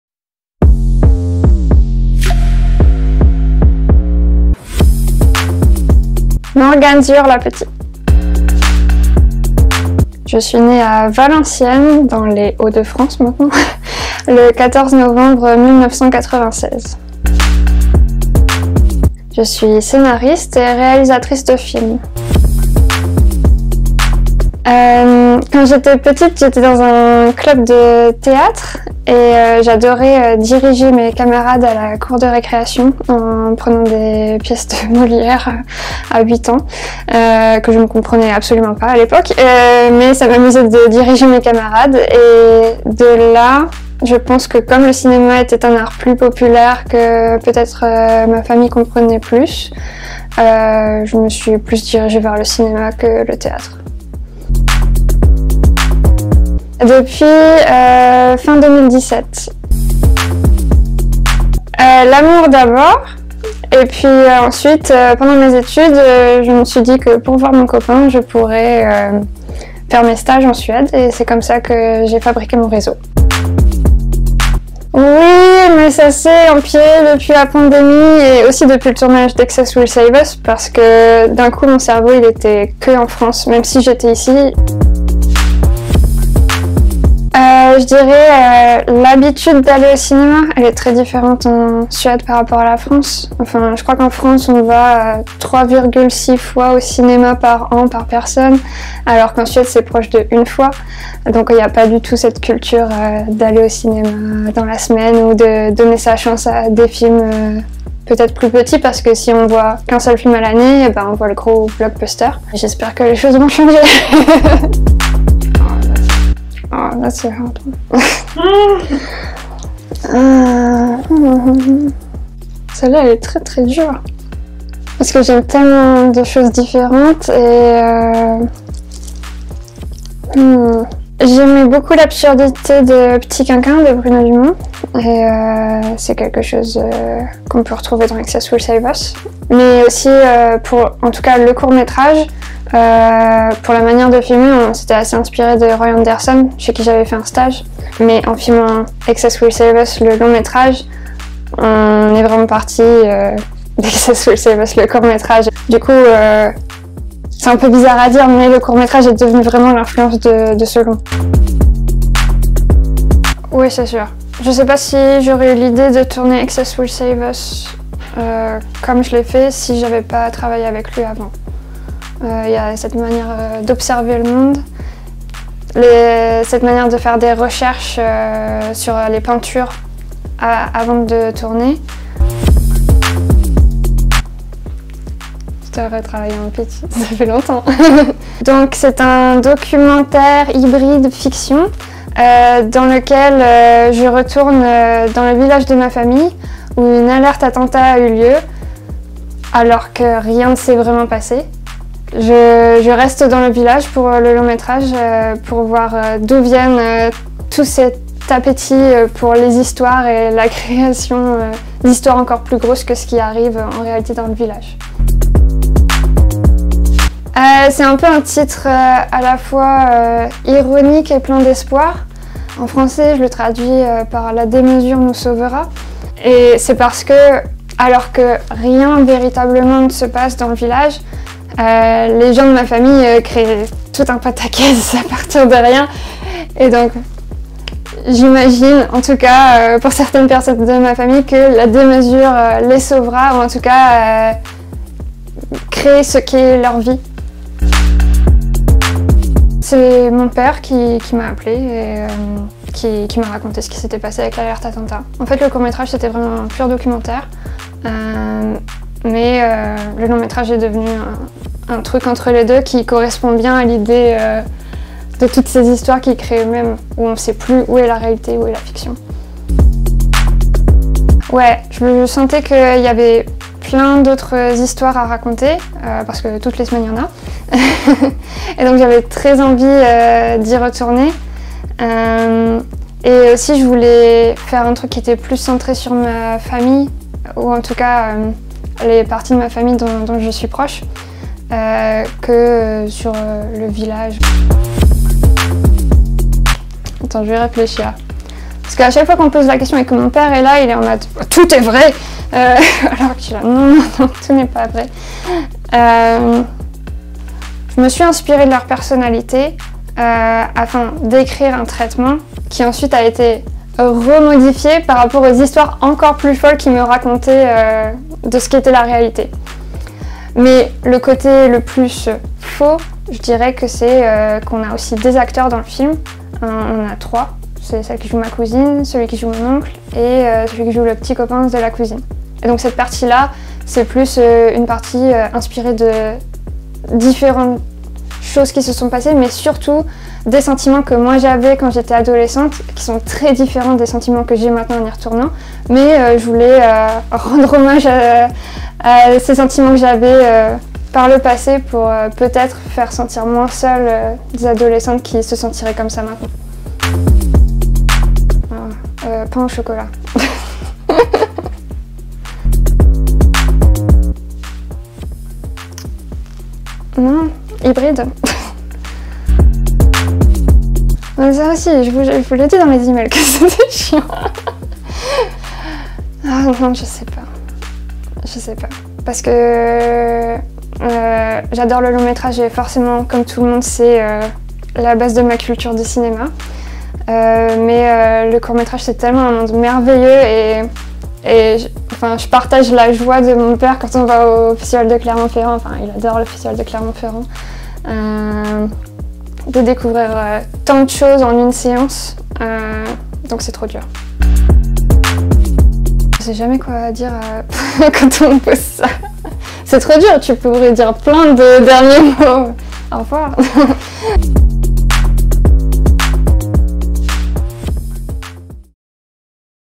La boîte à questions - Morgane Dziurla-Petit
Rencontrez Morgane Dziurla-Petit, réalisatrice de "Excess Will Save Us" (2022).

En avril dernier, l’IFS a eu l’honneur de s’associer au distributeur suédois Folkets Bio pour soutenir la sortie en salle de Excess will save us, le premier long-métrage de la réalisatrice française installée en Suède Morgane Dziurla-Petit.

Dans ce petit bijou de film, qui gomme les frontières entre documentaire et fiction, Morgane Dziurla-Petit explore avec humour, tendresse, violence, et aussi surtout avec une maturité déconcertante pour son jeune âge, la vie de sa famille et des habitants du village de Villereau, où elle a grandi. Un village situé dans le nord de la France, où rien ne se passe et où tout peut arriver…
Un ovni de film déjà récompensé par le Prix spécial du jury du Festival du film de Rotterdam.

Nous avons eu le plaisir de discuter avec Morgane à l'occasion de la Première de “Excess will save us” au Cinéma Zita le 1er avril 2022.
Retrouvez ici cette interview exclusive sous forme de boîte à questions !

Morgane Dziurla-Petit, née en France, est installée en Suède - où elle a notamment travaillé avec Roy Andersson - depuis 2017. Elle a précédemment réalisé et scénarisé trois autres courts métrages : The Day (2018), Excess will save us (version courte, 2019) et Grab Them (2020), qui ont reçu un accueil international et ont été nominés pour plusieurs prix du court métrage.
Elle est l’une des lauréate 2022 de la bourse mobilité de l’Institut français de Suède, qui lui permettra de préparer son prochain long-métrage (sortie prévue en 2024).

Pour plus d'information sur Excess will save us, rendez-vous sur sur le site de Folkets Bio